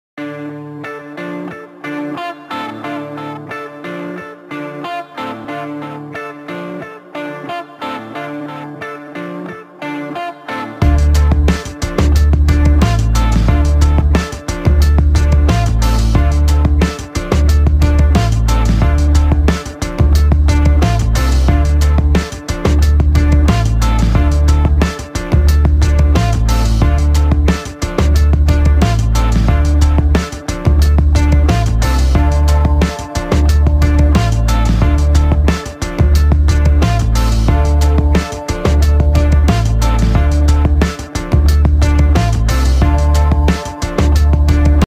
Ye hai jalore ka hal abhi 9:40 am Thursday ki news he
Sabhi ko share kare